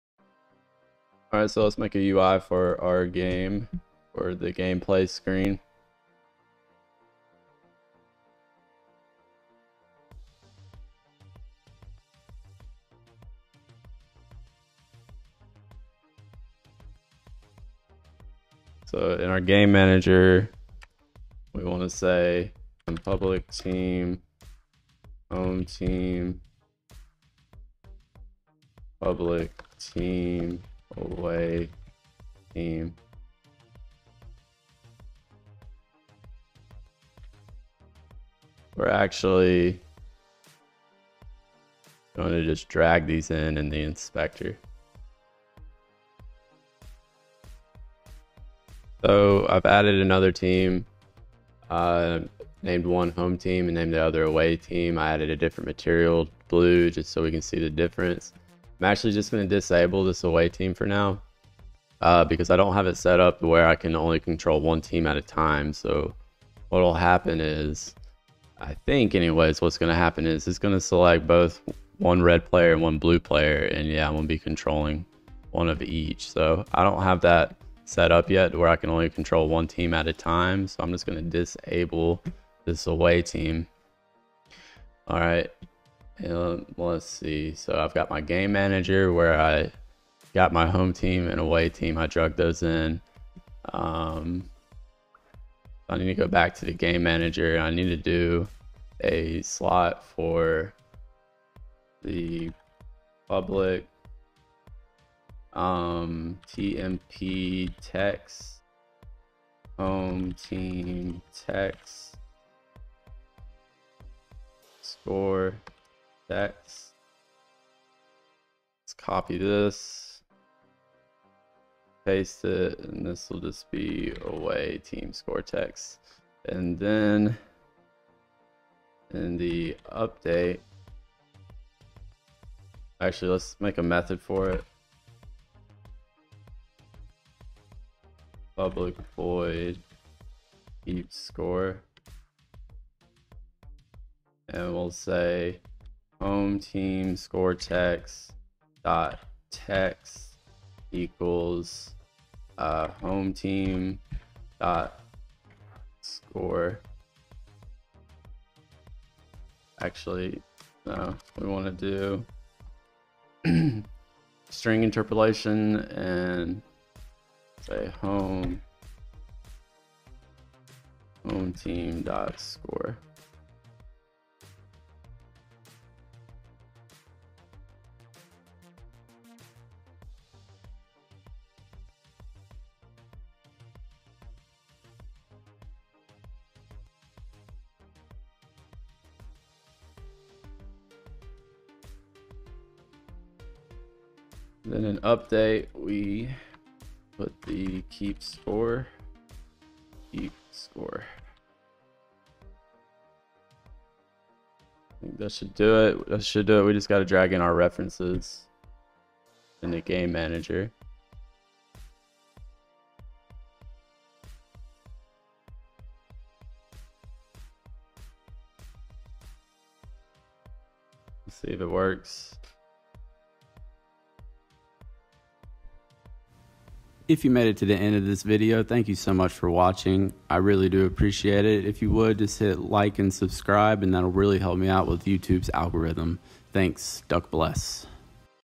Score UI || Unity Soccer Game
See my NEW 2024 Series on How to Make a Soccer Football game in Unity HERE:

How to make a working UI for the score of the soccer game.